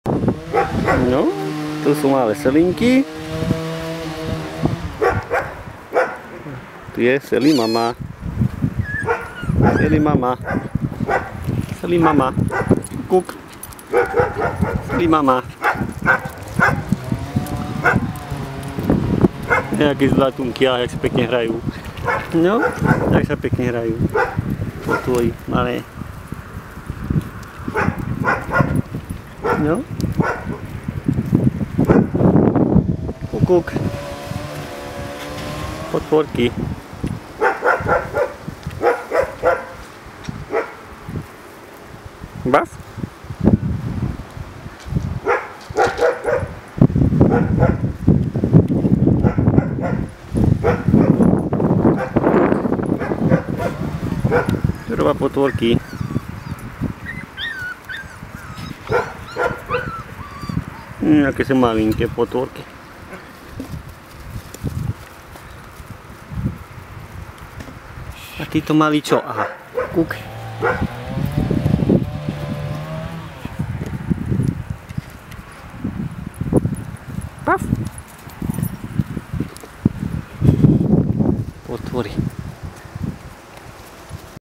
Lukov dvor 452 Malé Selinky 6 .10 .2012
Tu si môžte pozrieť malé šteniatka od labradorky Seliny, od bývalého suseda.6.10. 2012